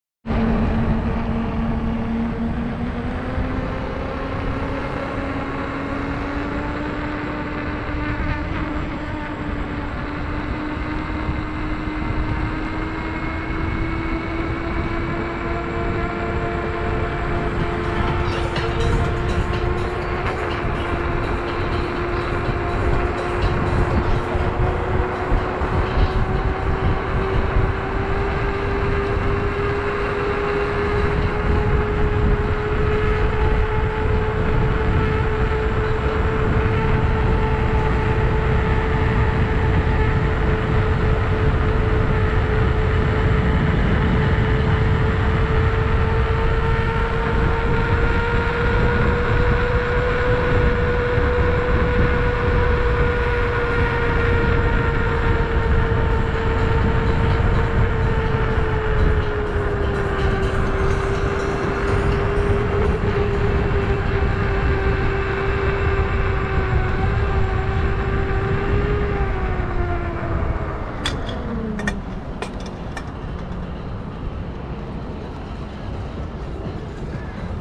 Veteran Sherman & TSG Pass - sitting riding on EUC
Amazing feelings and awesome looking when you riding sitting on Veteran Sherman!